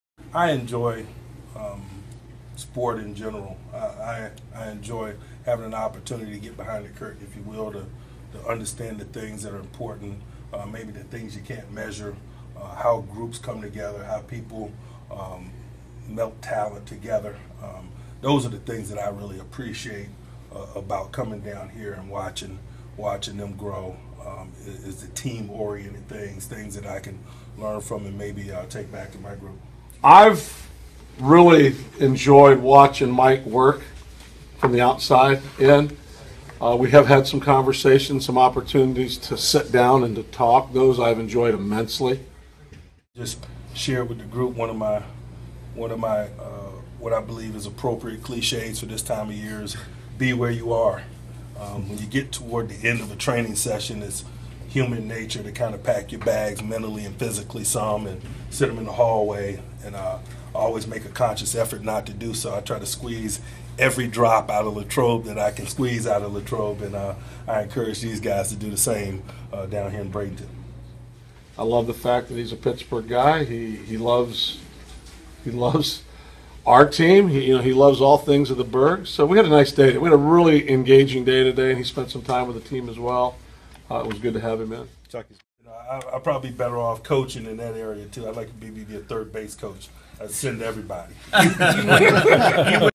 Steelers coach Mike Tomlin visited the Pirates on Monday and discussed his bond with the team as w
Steelers coach Mike Tomlin visited the Pirates on Monday and discussed his bond with the team as well as Pirates manager Clint Hurdle.

By: John Lembo
Published on: March 29, 2016